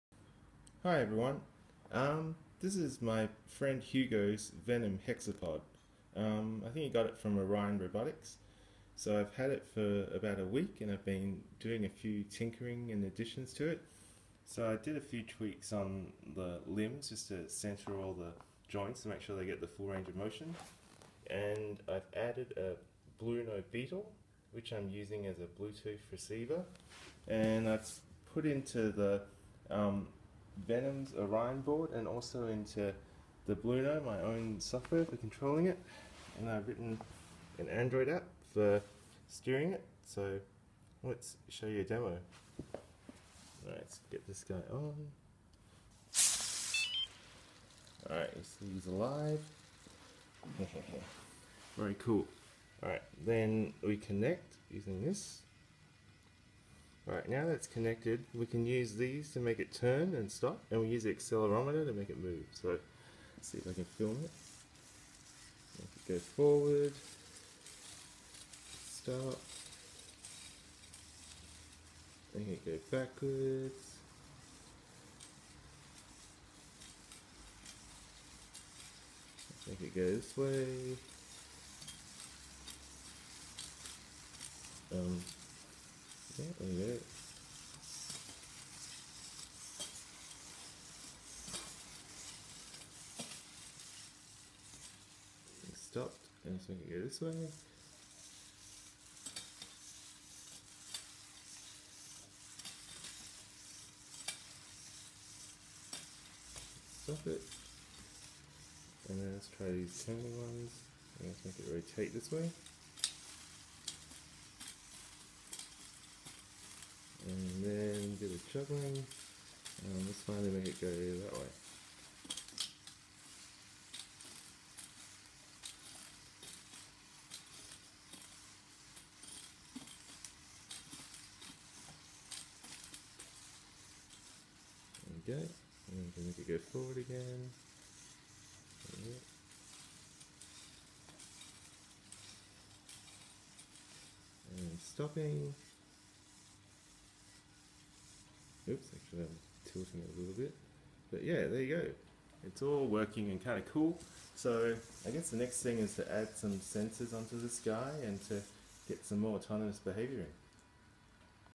Venom Hexapod - Bluetooth Control Demo
Demo of Bluetooth Controller app for my friend's Venom Hexapod (from Orion Robotics).

I've borrowed the hexapod for about a week, and have been making it controllable over Bluetooth using an app.